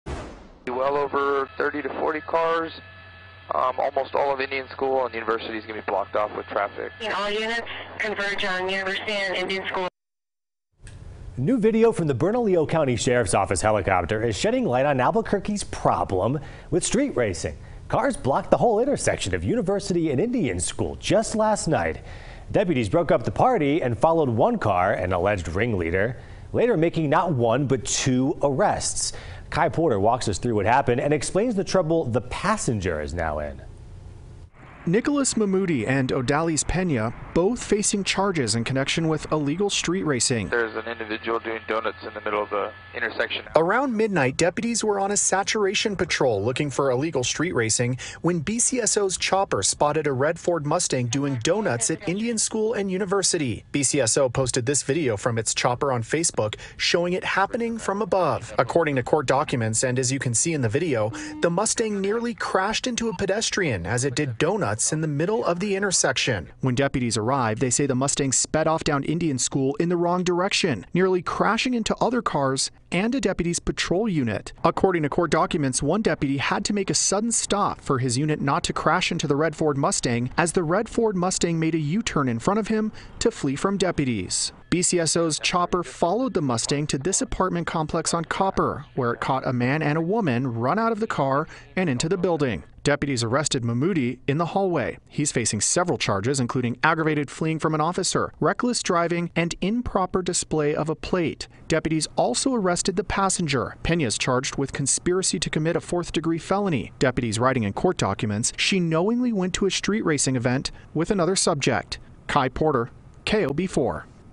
BCSO chopper video shows cars doing donuts in Albuquerque intersection
New video from the Bernalillo County Sheriff's helicopter is shedding light on Albuquerque's problem with street racing.